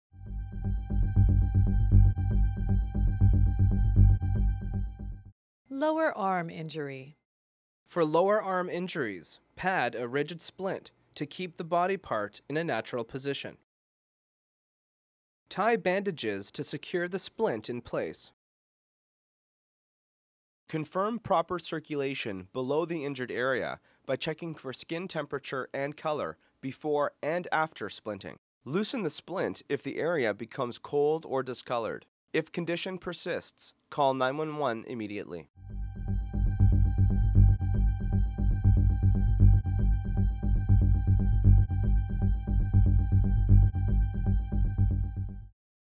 Arm Splints with Nick Rondinelli
How to apply a splint to an injured arm.